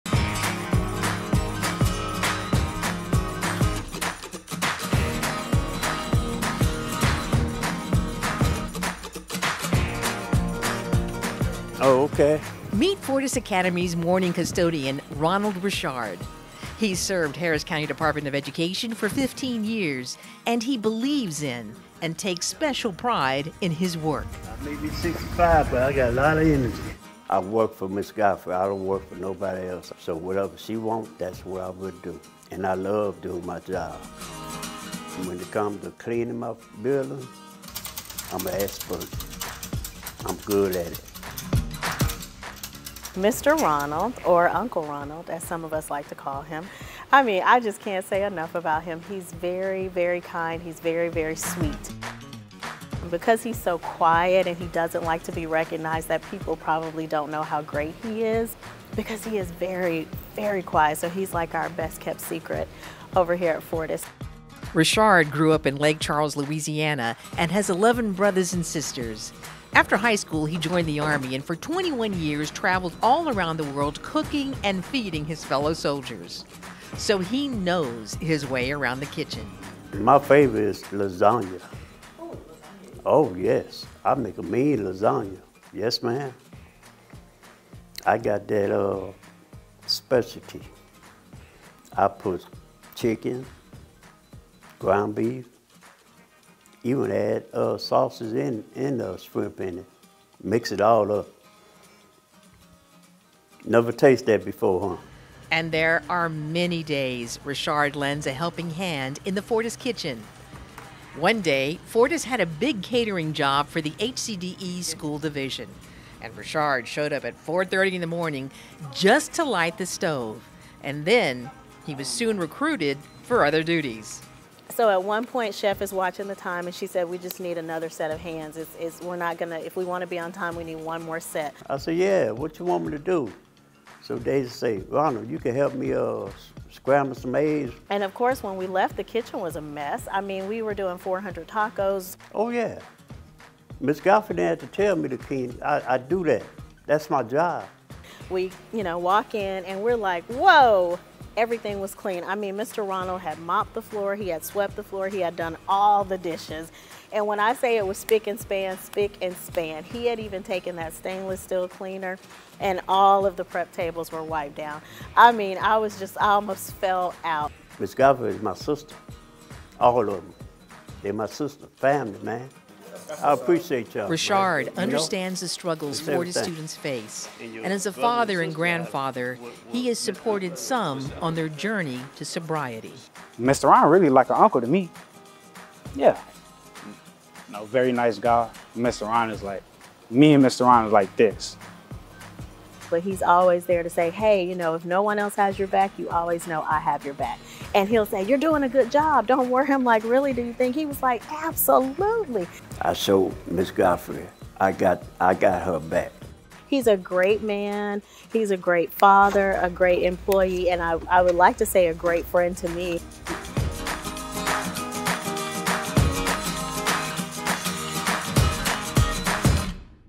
HCDE Employee Spotlight: Ronald Richard
Fortis Academy's morning custodian Ronald Richard goes above and beyond every day. He keeps the campus spotless and believes in making life a little easier for his principal, colleagues, and students.